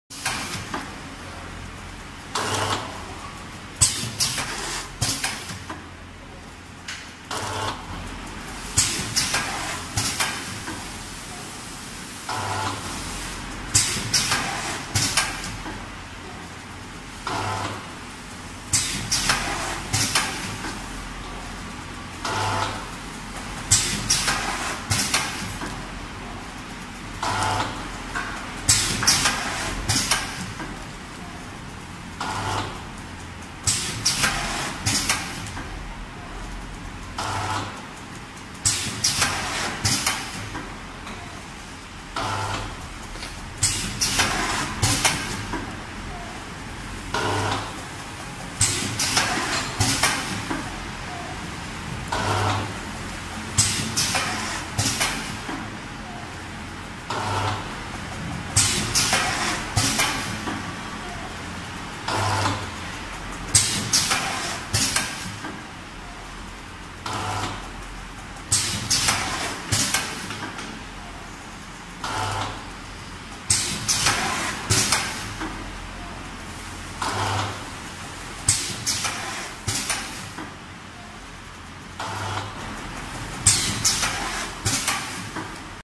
Wenzhou Kingpak HP200L for Paste Packaging machine
Wenzhou Kingpak Manufacturing Co.
HP200L for Paste packing
Filling: 100-500ml
 15-30 Bags/Min